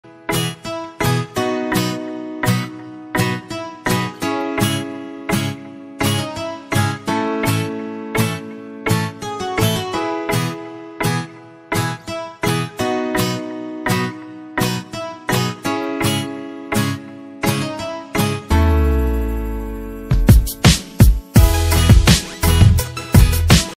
Still life painting - 2D ( Junior level) by Sai charan of class 3
Our student Sai charan working video of still life painting -2D